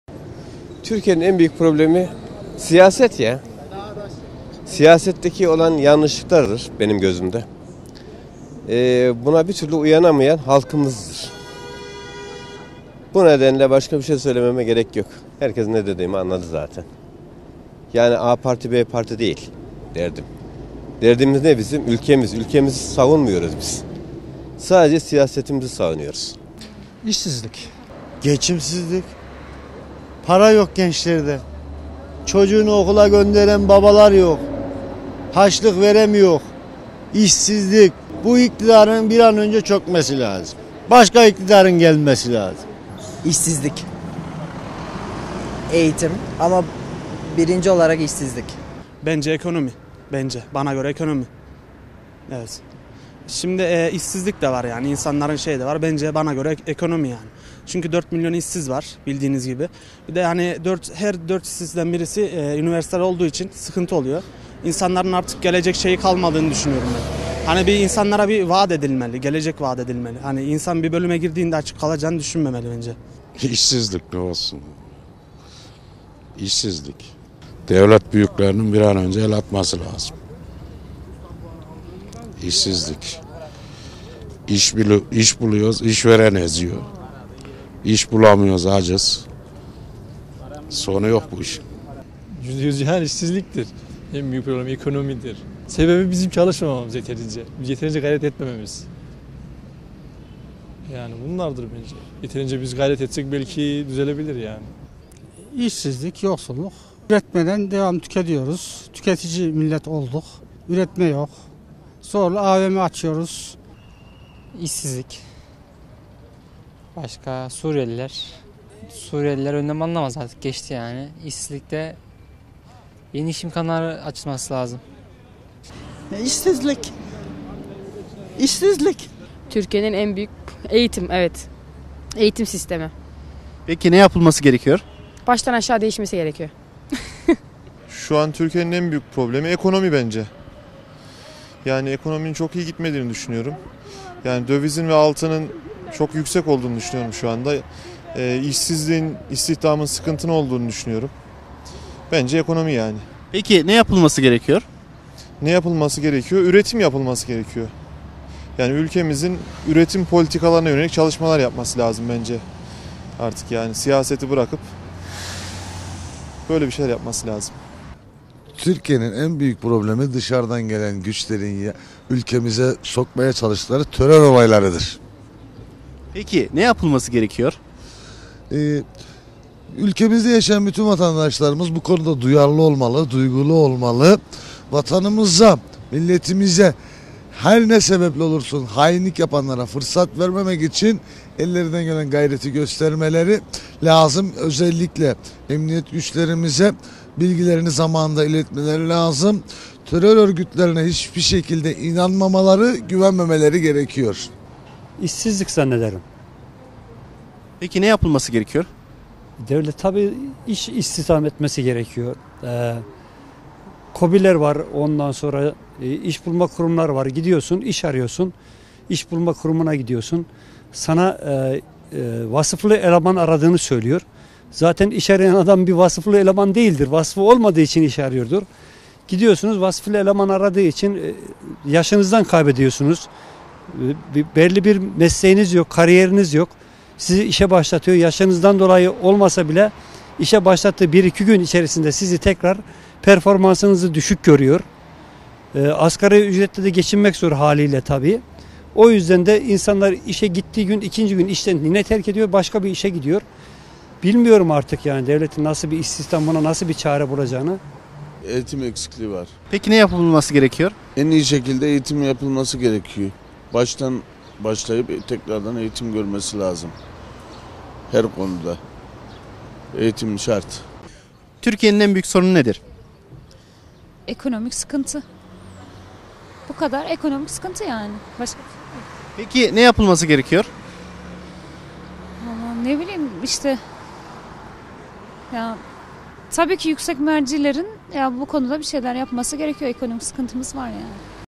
Sizce Türkiye'nin En Büyük Sorunu Nedir?
Laf Sokakta ekibi, bu kez, Türkiye'nin en büyük sorunu nedir diye sordu. Kayseri'nin nabzı, Kayseri'nin cevapları..